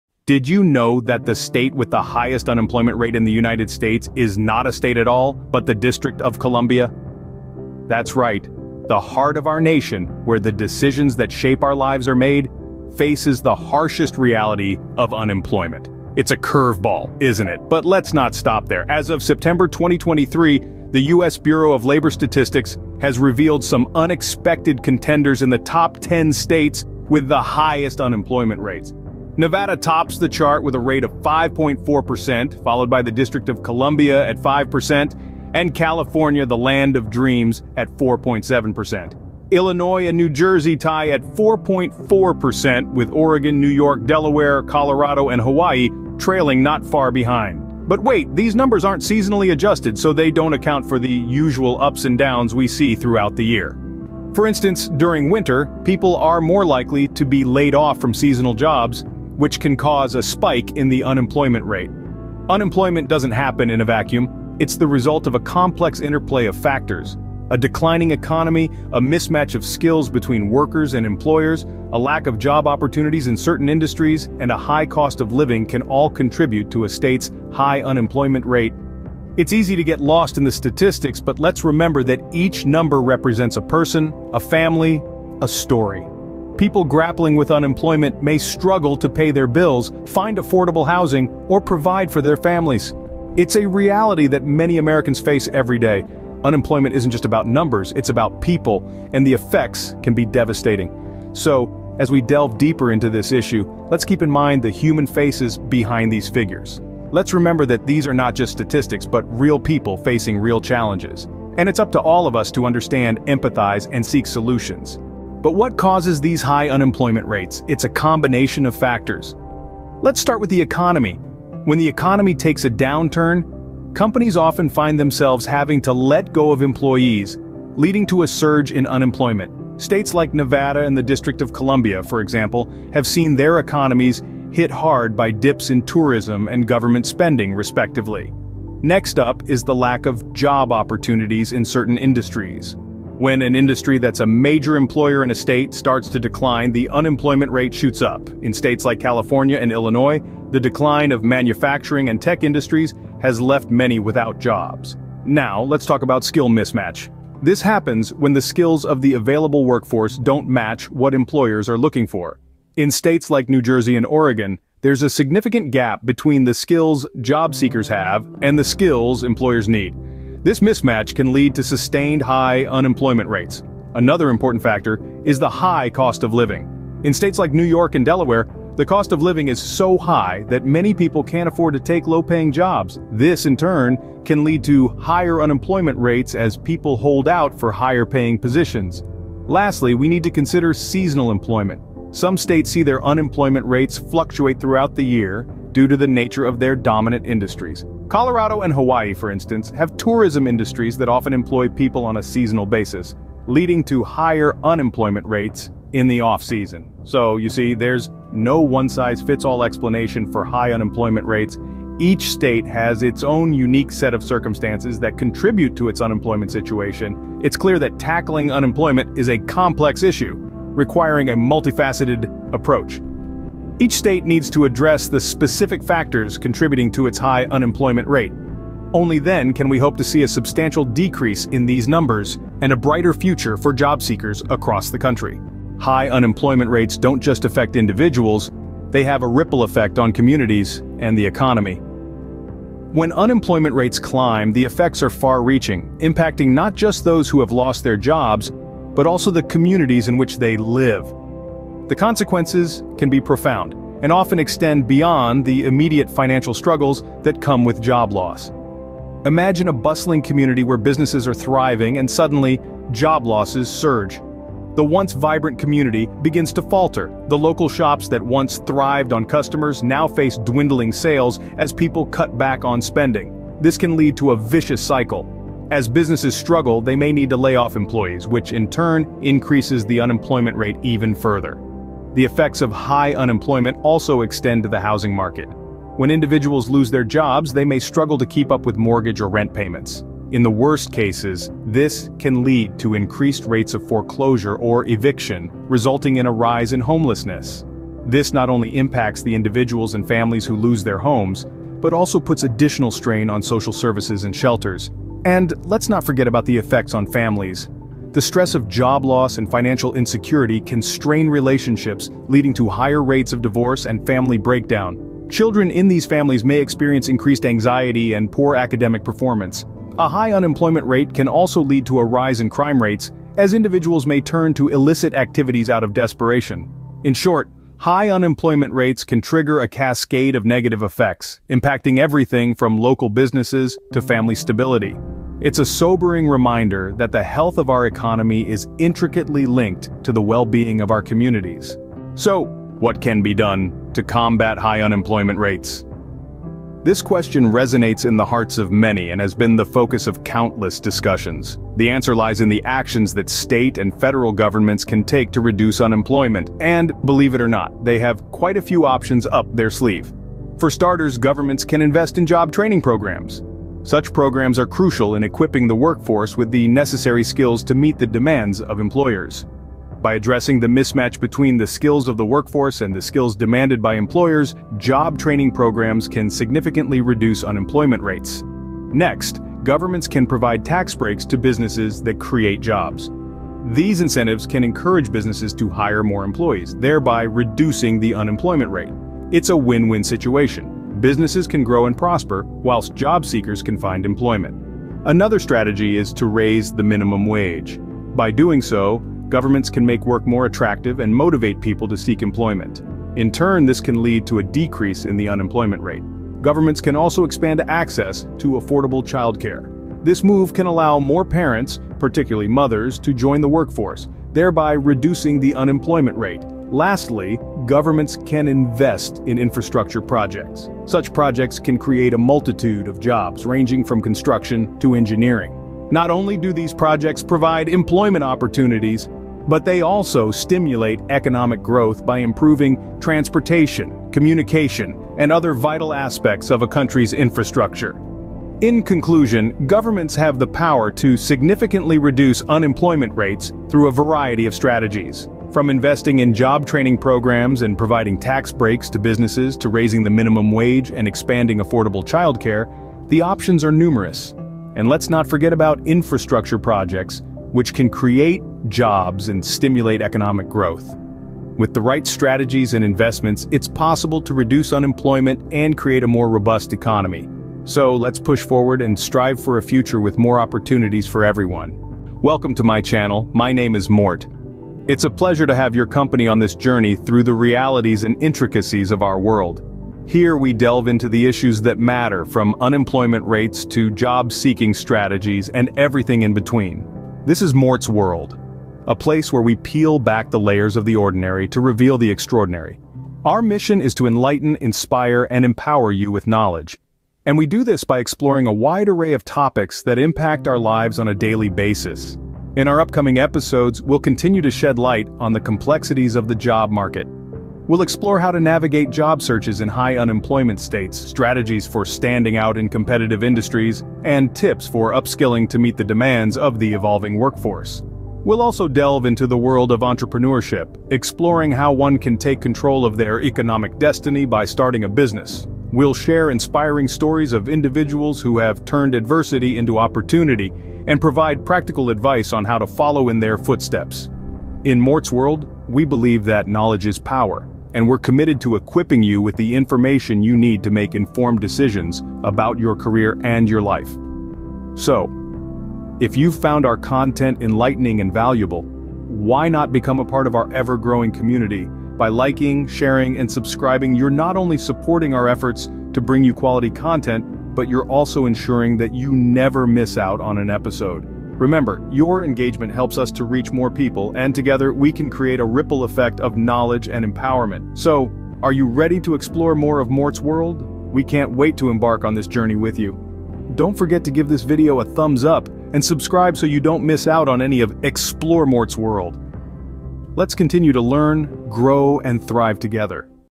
10 States with Sky High Unemployment
In this eye-opening video, we delve into the top 10 states with the highest unemployment rates in the U.S. 📊 Discover the shocking statistics and unravel the reasons behind this economic turmoil. 📉 From job market fluctuations to economic factors, we leave no stone unturned. Join us on this informative journey as we shed light on the root causes and potential solutions to combat unemployment in these struggling states. Don't miss out on this thought-provoking analysis! 🔍💼

OUTLINE:
00:00:00 The Unexpected Reality of Unemployment
00:01:55 The Causes Behind High Unemployment Rates
00:03:55 The Effects of High Unemployment Rates
00:05:41 How Governments Can Help
00:07:51 Welcome to Mort’s World